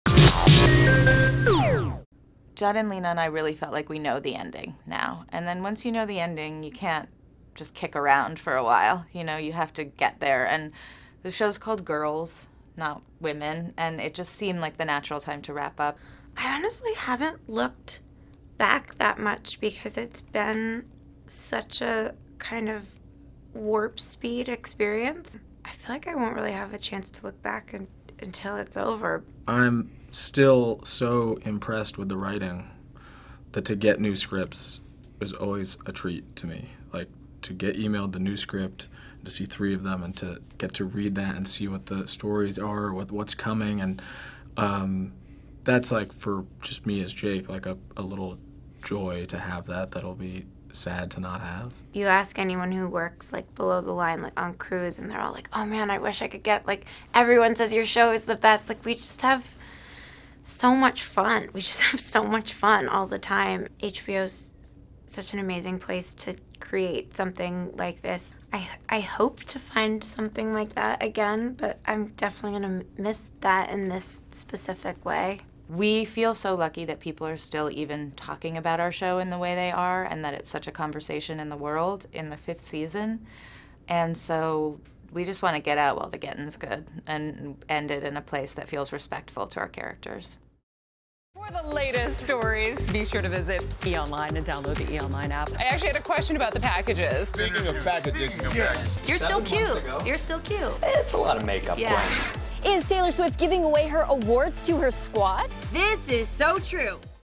"Girls" Cast and Producer Gush Over Series Ending | Celebrity Sit Down | E! News
Jenni Konner, Zosia Mamet, Alex Karpovsky and more reveal their thoughts on season 5 of the HBO series. Get the scoop on the end of "Girls."